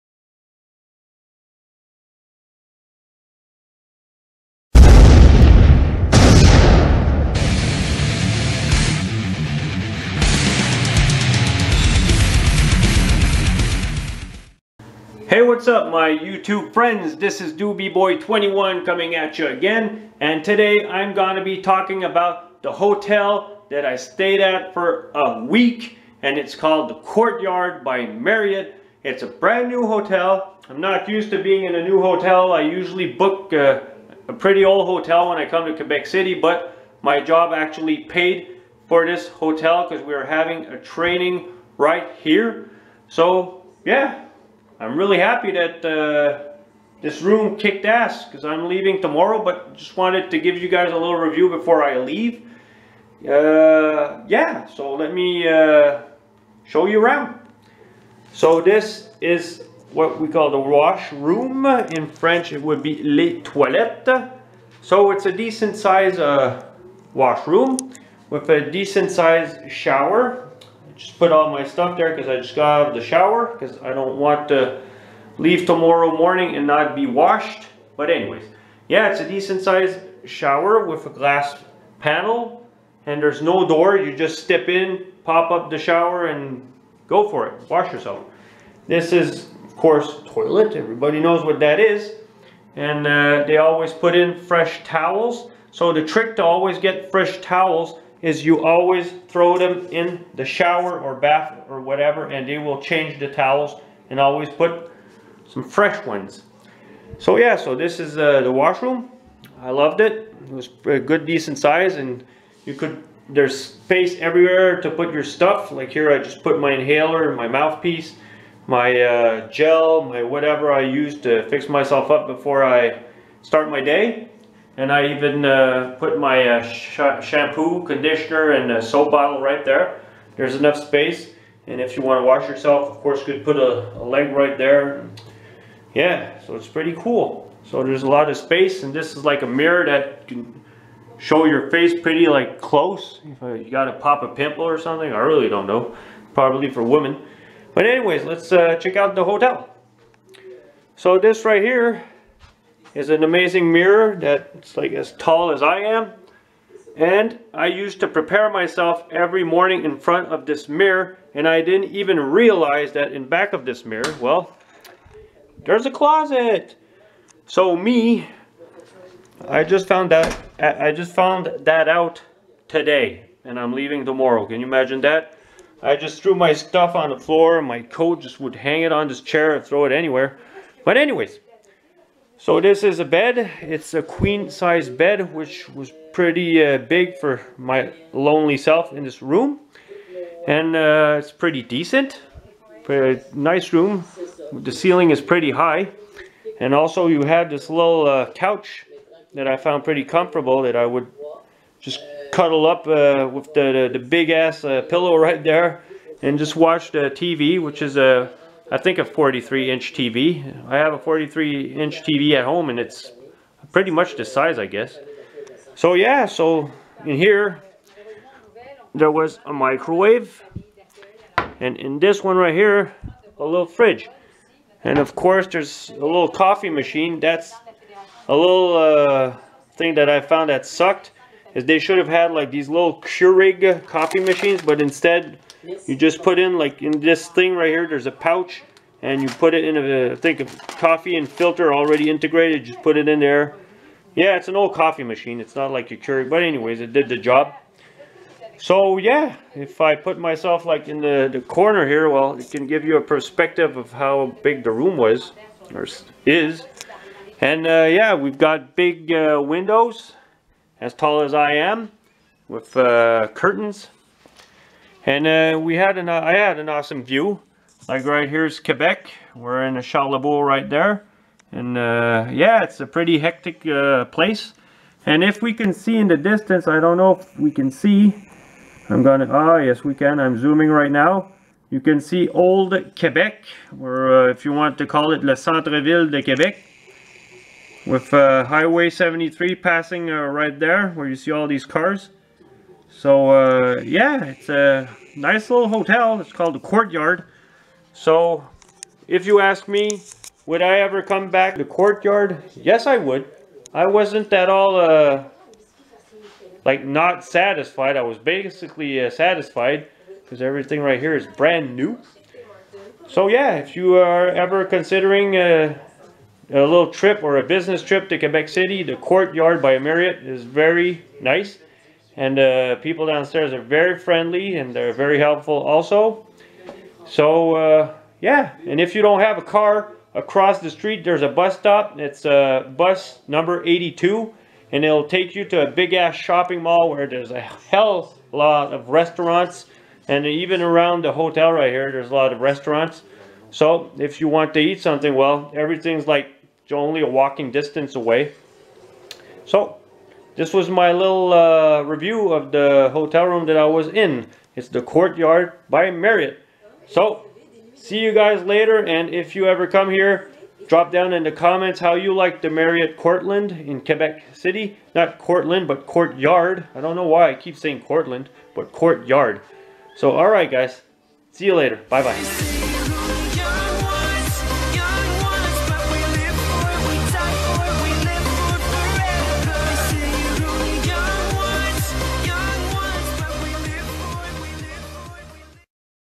Courtyard Hotel Quebec City Review
Hey guys ! Decided to do a review on this hotel which is pretty new in Quebec City ! Check it out ! :)